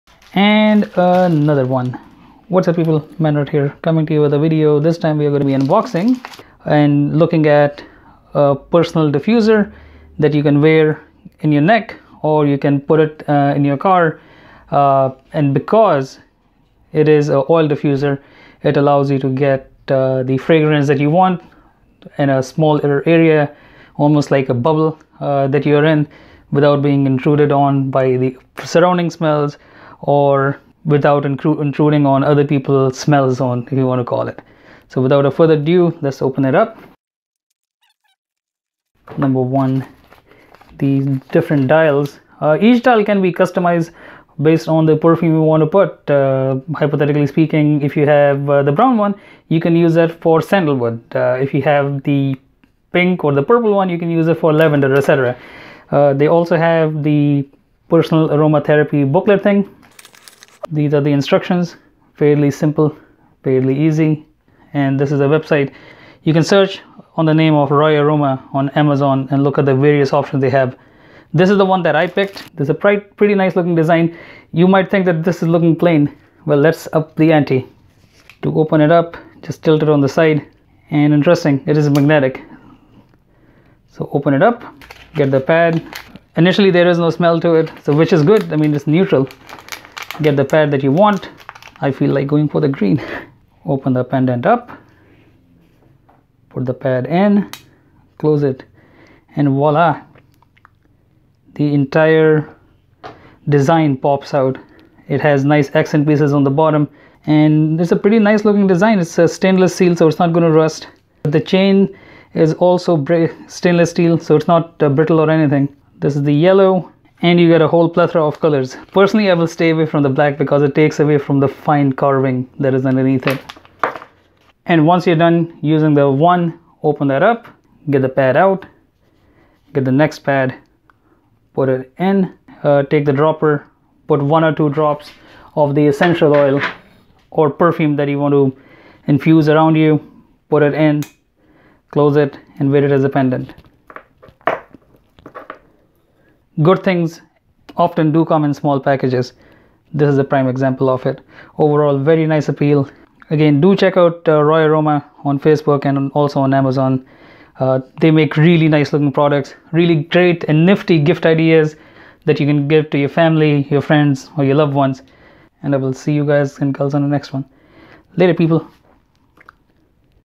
Royaroma Personal Essential Oil Diffuser
This is a very nice and elegant looking pendant. Makes for a great gift idea that not only can be customized with various color options but also can be used as a personal essential oil diffuser. Very nice gift for family and friends. If you don't want to wear it, put it in the car or in the office as an accent piece. Makes for a great conversation starter for sure.

Lasting aromatherapy diffusion:Small holes on the locket to enhance diffusion.Natural scent of essential oil lasting longer than perfume.No longer need to stand the chemical freshener
Convenient to use essential oil:This is a beautiful antique diffuser necklace to wear your essential oil anywhere you prefer not depending on the place anymore
Friendly to allergic individual: It’s good for those who has sensitive skin and can’t use oils on their skin due to allergies. The necklace diffuser gives you a way to wear essential oils. And It’s good for migraines， headaches，sinuses
Quality material:Surgical grade 316L stainless steel for lifelong durability.Easy opening with a strong magnet for closing.Nature rubber on the locket to prevent scratch.Fairly sturdy chain and a nice heavy weight.
Easy to use:Open the locket and put the felt pad inside it.Then add a few drops of your essential oil on the felt pad.Wear it on your neck and you will get the great fragrance.

Take Care